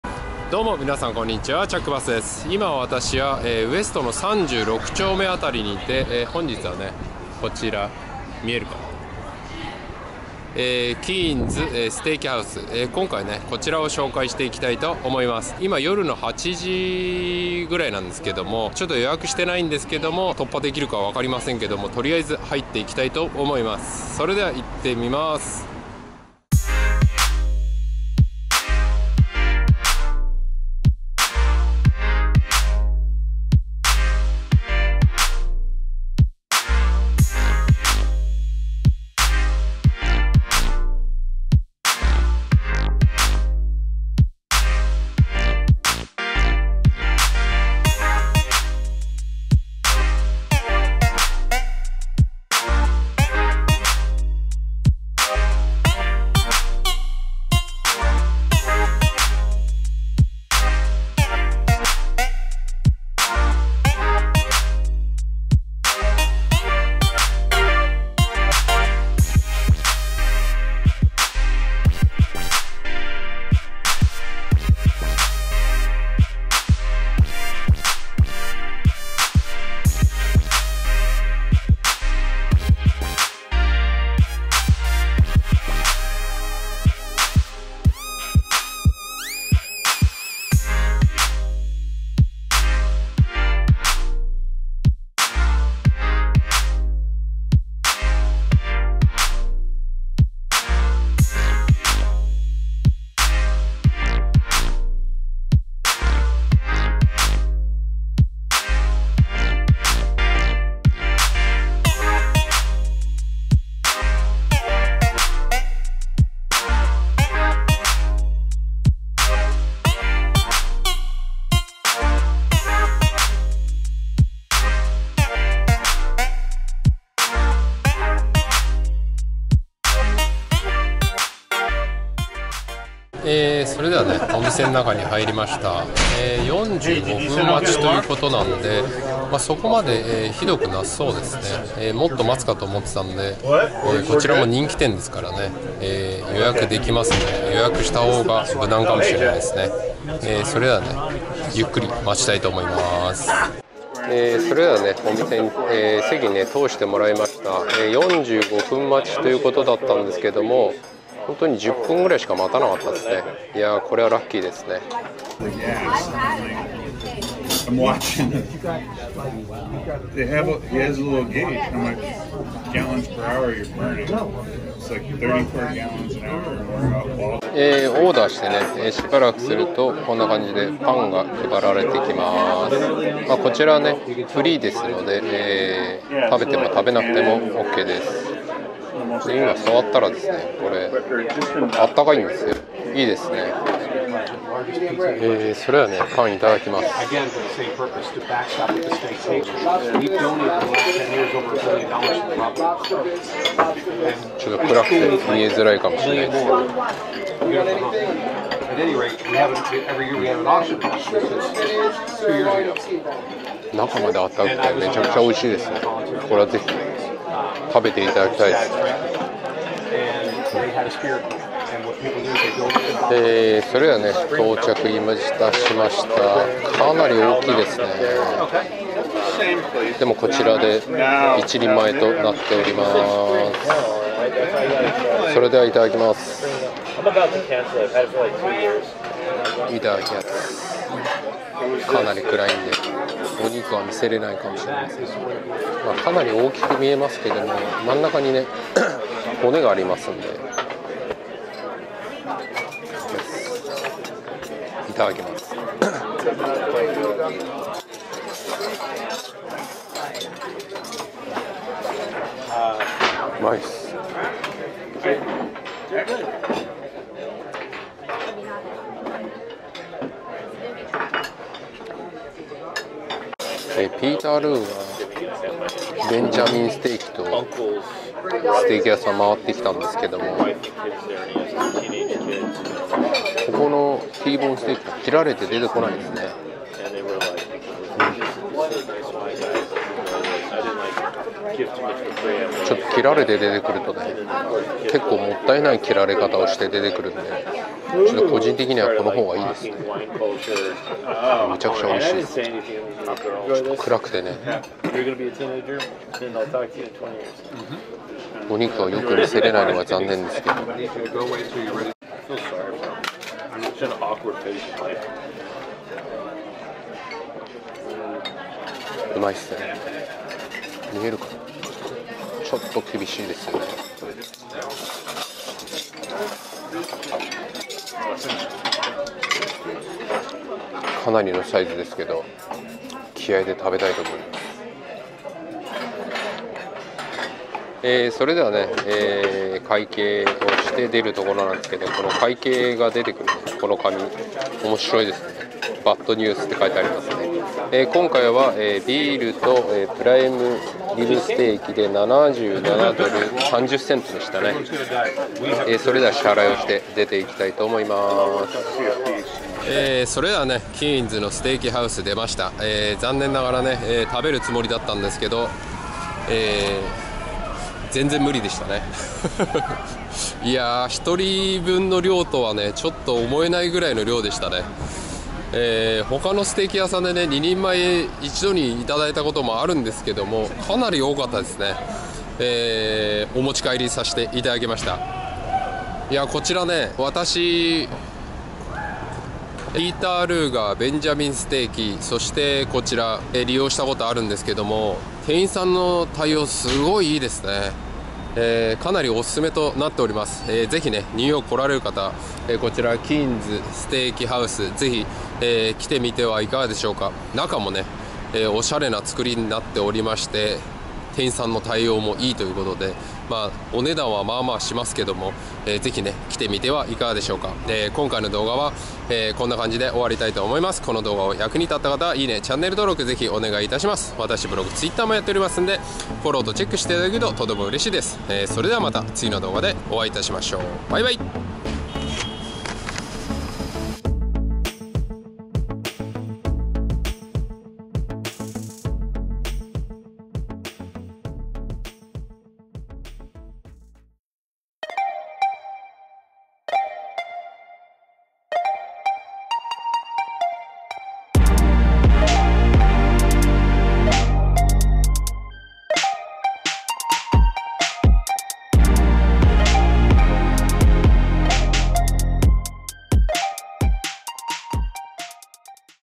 【ニューヨーク観光・旅行】ニューヨーク旅行にオススメ!!有名店キーンズステーキハウス[keen's steak house]を紹介！ニューヨークステーキハウスならここで間違いなし!?
ニューヨークマンハッタンにあるステーキハウスキーンズステーキハウスを紹介！味は美味しく人気店なのに店員さんの対応もよかったお店です！

キーンズステーキハウスのブログ解説はこちらです！予約方法、アクセス方法などはこちらを参考にして下さい♪

スマートフォンアプリ　オープンテーブルで予約可能です♪

・撮影機材

企業の方へ
私はニューヨークで働けるお仕事やツアーなどを募集しています。ありましたらぜひお声がけお願いします。

ニューヨーク観光・旅行